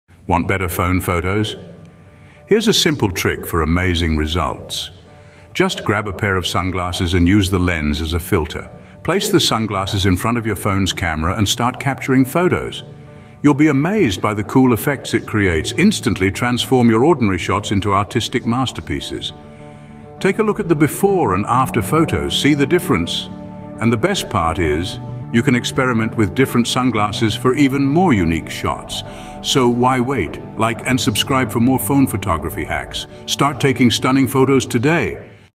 45 Second Phone Camera Trick for Amazing Photos!
Upgrade your phone photography skills in just 45 seconds! 📸😎 Discover how a simple pair of sunglasses can transform your mobile photos into artistic masterpieces. This quick hack shows you how to use sunglass lenses as filters, adding a unique flair to your shots. It's easy, innovative, and works with any smartphone camera. Watch our video to see the stunning before-and-after results and experiment with different sunglasses for varied effects. Perfect for aspiring photographers and social media enthusiasts.